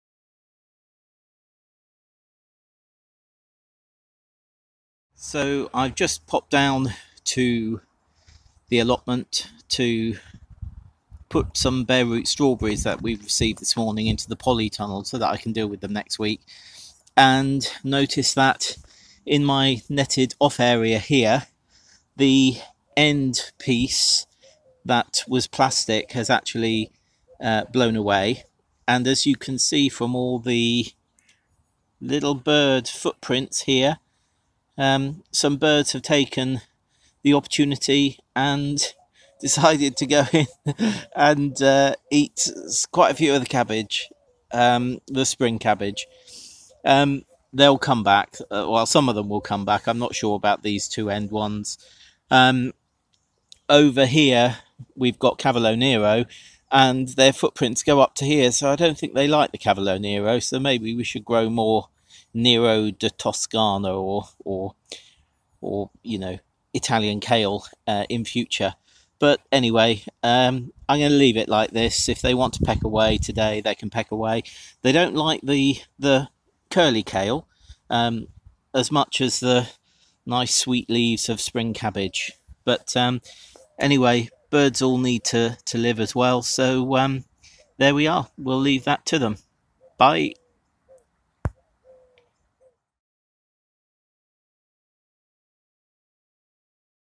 Hungry Birds and Poor Netting with Paul, March 2018
Paul discovers the birds have had a feast.